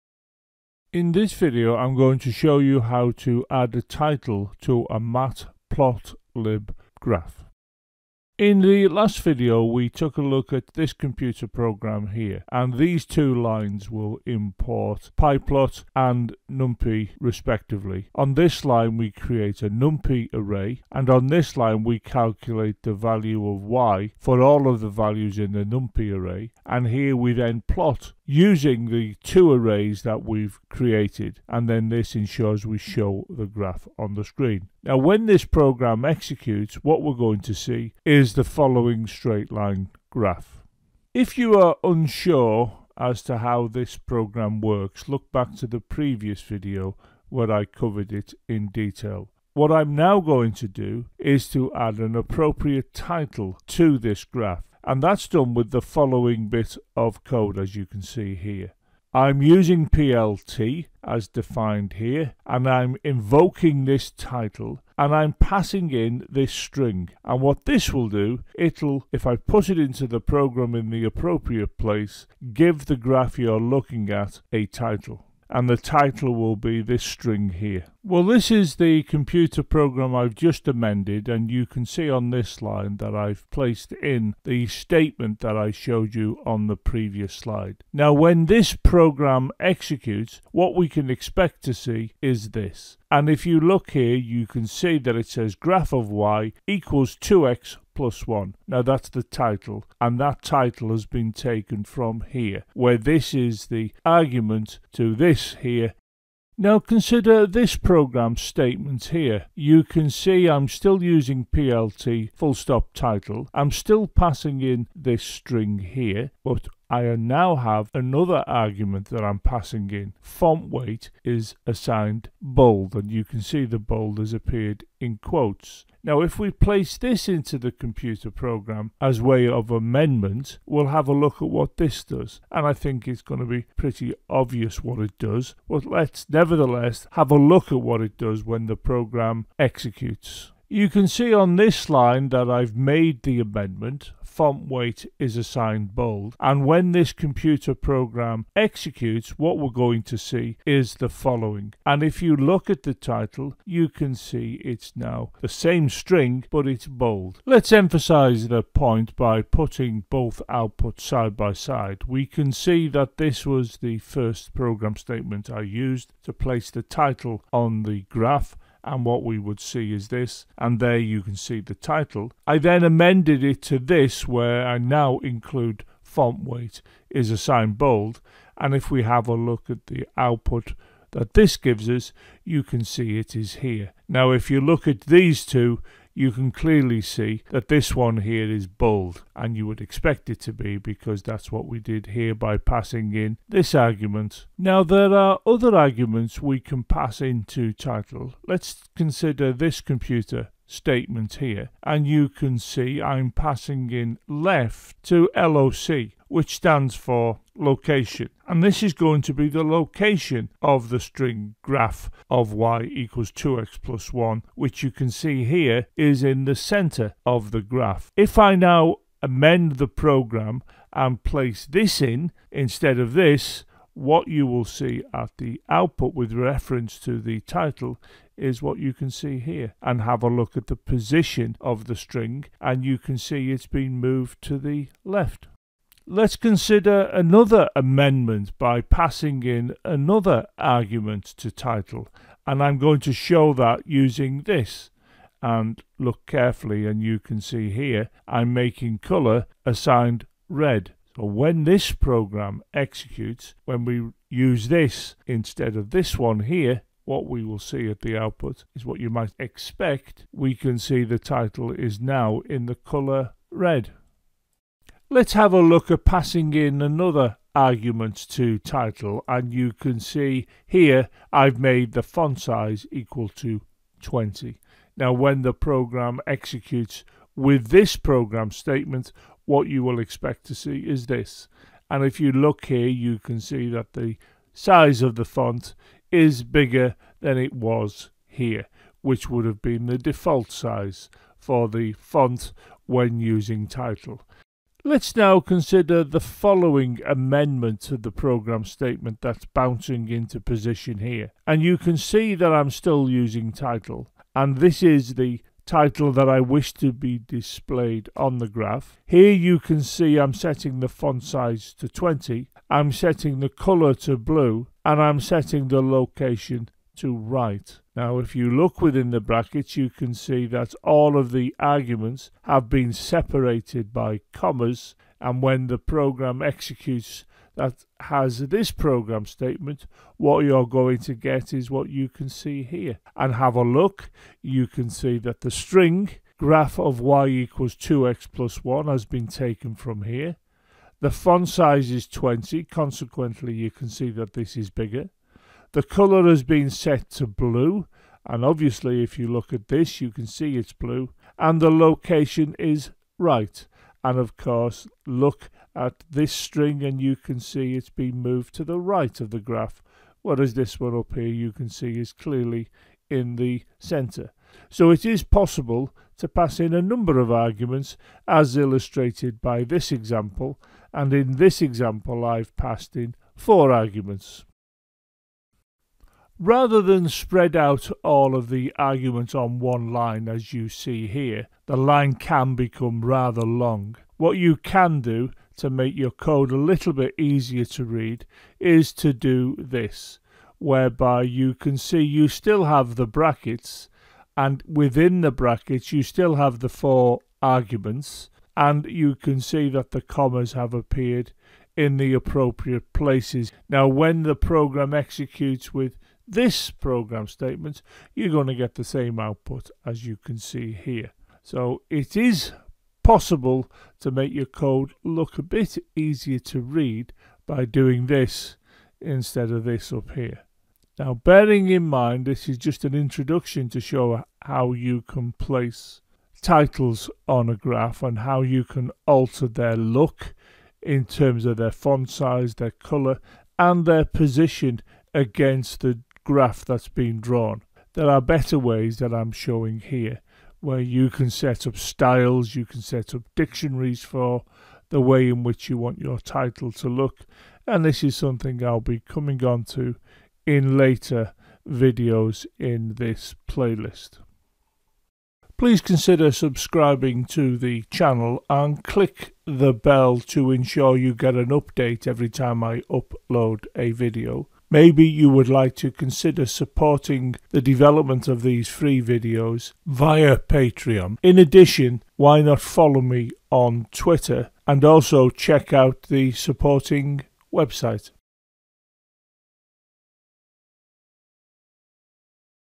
Adding a title to a graph plotted using matplotlib
In this video I show how to add a title to a graph that has been plotted using matplotlib.